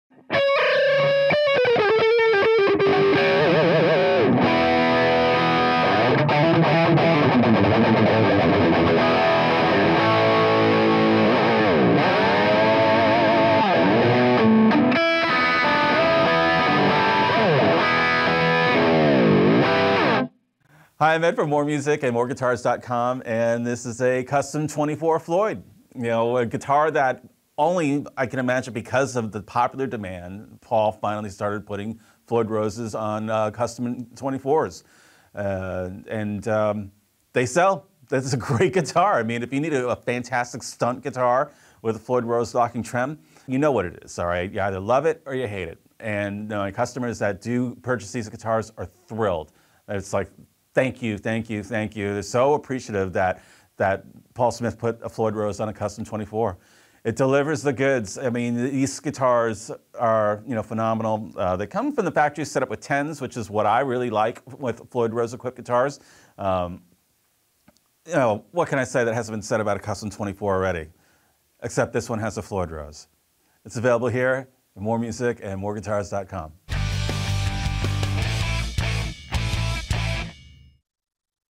Custom 24 Floyds Are Back! (PRS Core Model Shred Machines!)
It hasn't been easy to find American-made guitars with high-end Floyd Rose trem systems without going the Custom Shop route. When PRS began producing the Custom 24 Floyd, players looking for a high-end American solution were thrilled. Getting a hold of one... now that was another challenge.
They've always been in high demand, and whenever we get them in - they're gone fast! And post-pandemic, finding one has been as rare as sighting Sasquatch piloting a UFO.
But they're back, with all the 2023 PRS Custom 24 features, and we've got 'em!